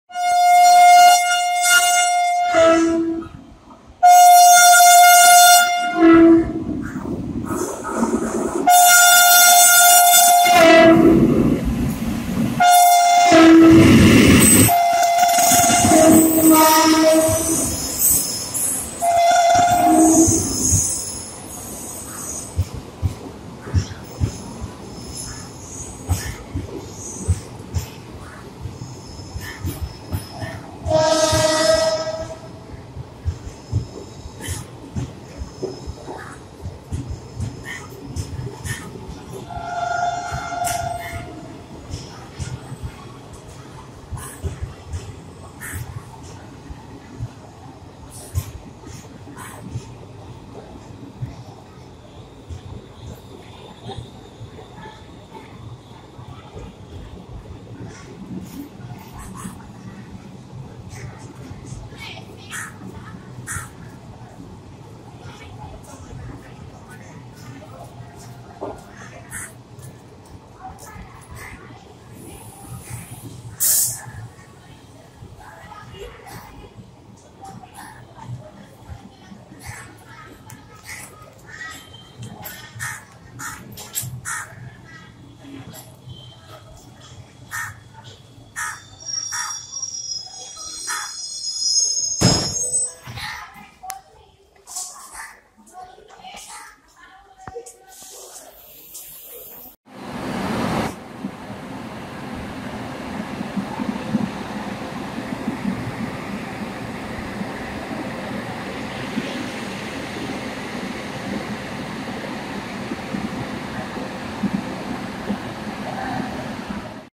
LGD WAP 7 at Hubballi: Vijaywada Hubli Amravati express
E Loco at Hubli: RPM WAP-7 hauling Hampi express

Hubballi electrification, CRS Inspection Hubballi, E-loco Hubballi

to get News and Updates of Hubballi Dharwad